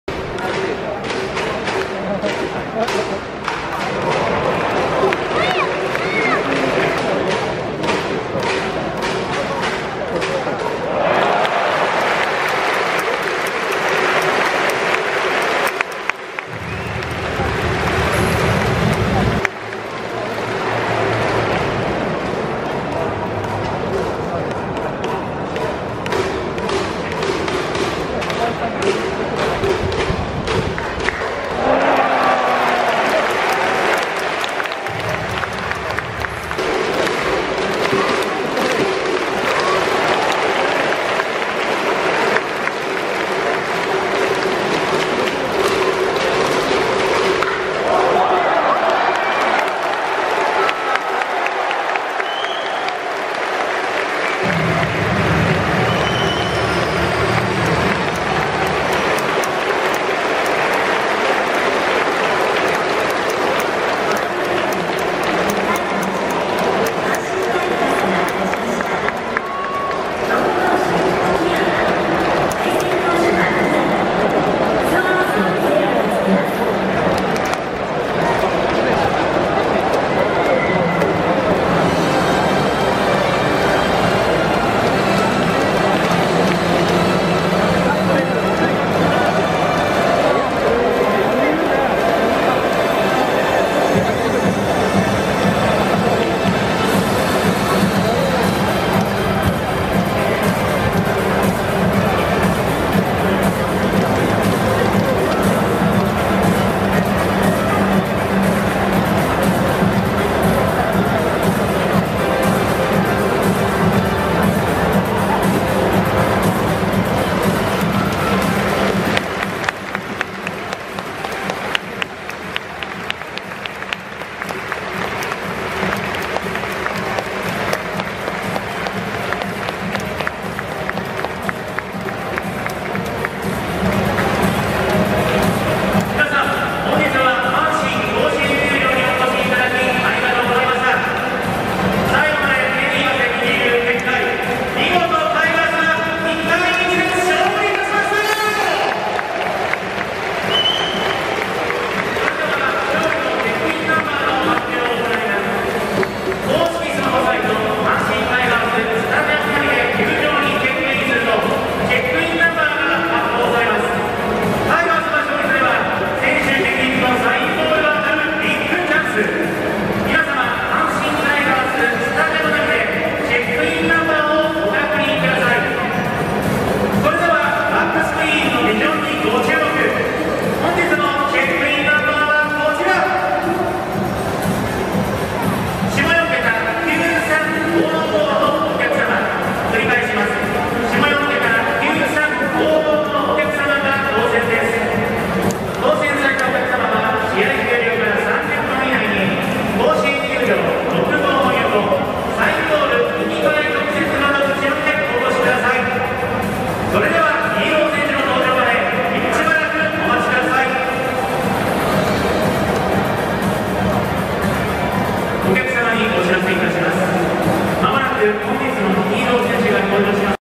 スアレスがとらほー！157キロがえぐい｡打てない！勝利の瞬間 2021-6-2
スアレスが登場！勝利の瞬間！
スアレスが出てきたら打てない！

マルテ9号ホームラン！井上ヘッド､佐藤､中野ベンチみんなでラパンパラ！ベンチが明るい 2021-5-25